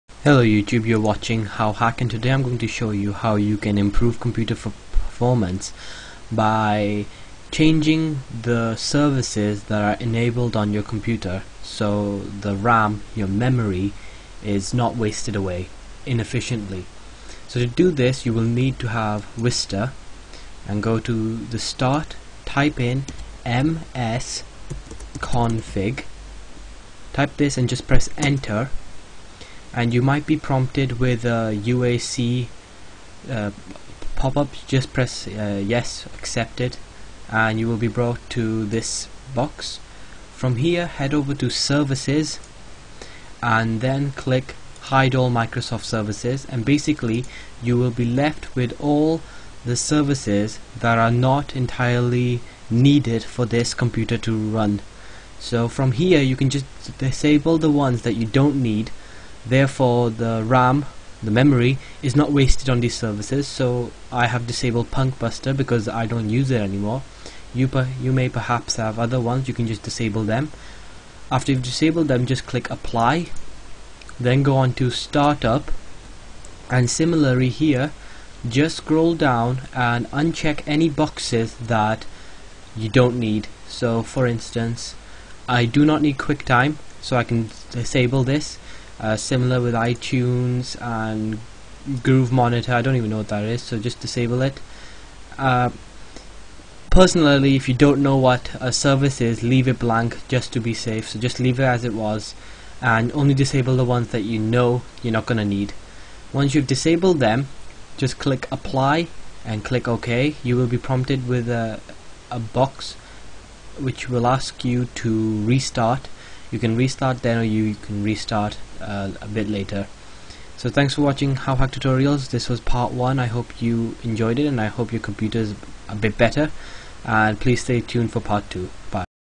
Speed Up Computer - Decrease Gaming Lag FOREVER!
How to speed up your computer in seconds and decrease any type of lag!

PART 1